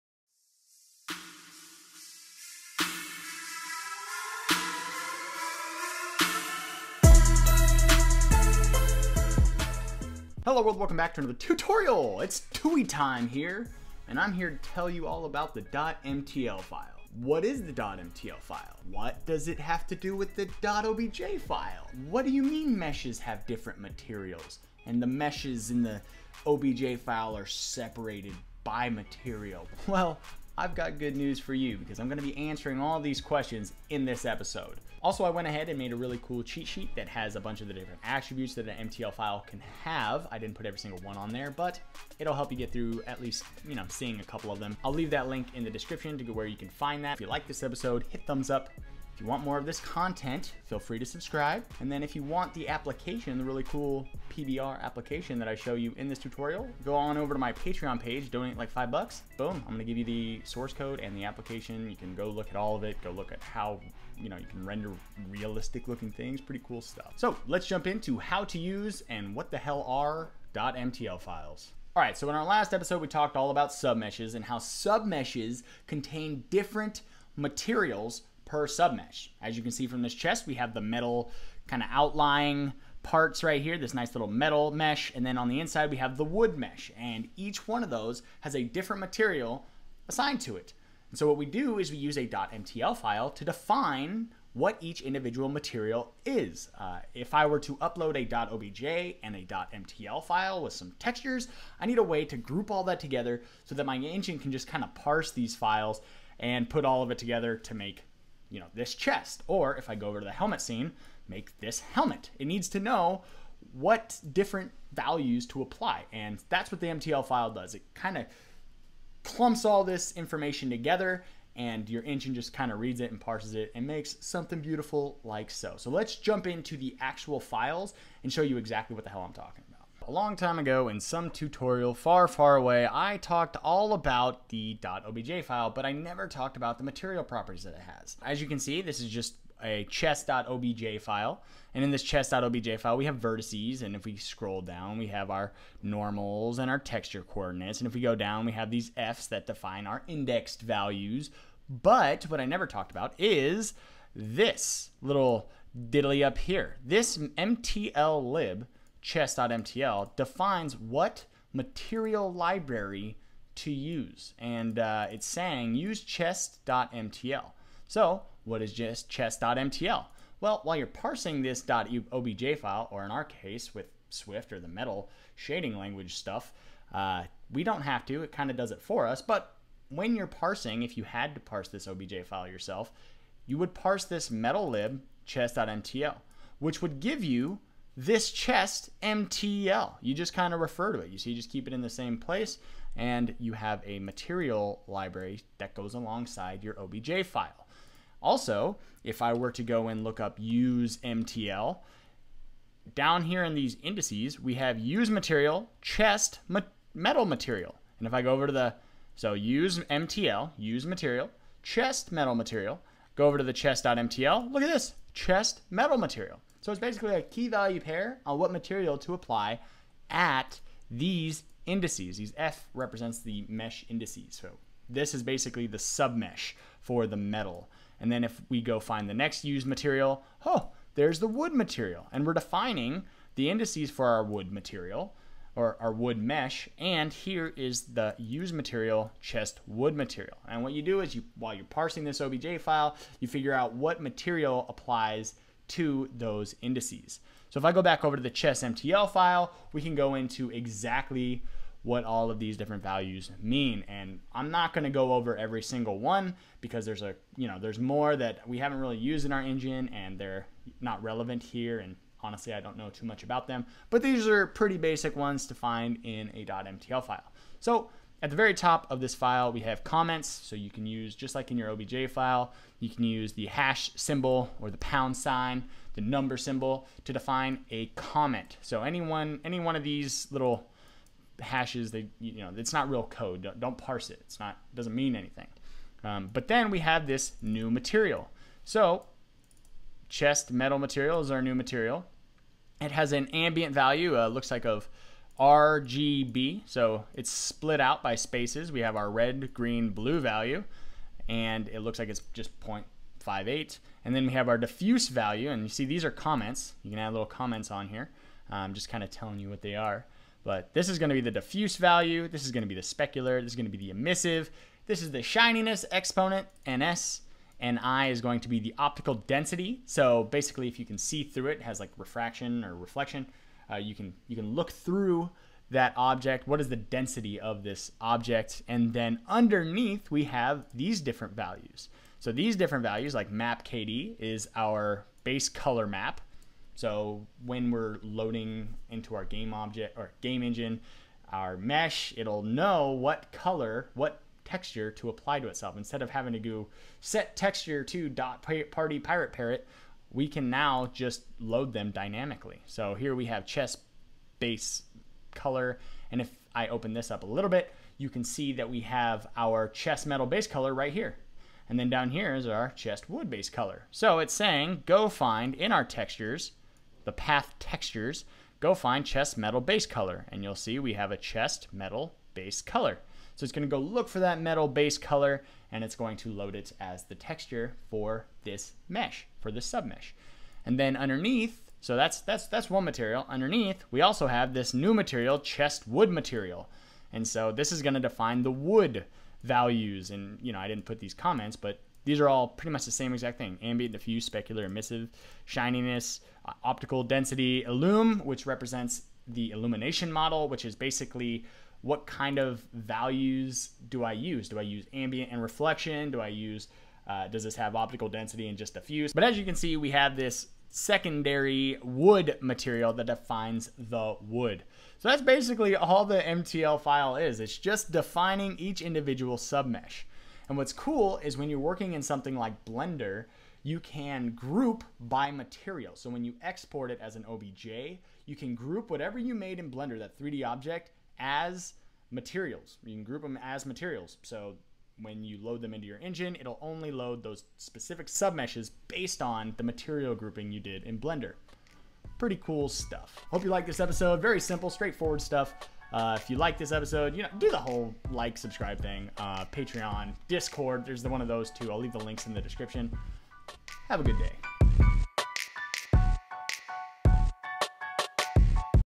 31: What is a .MTL File?
:In this episode I will go through exactly how a .MTL file works with your graphics projects and why they are extremely important in creating really cool graphics, really fast.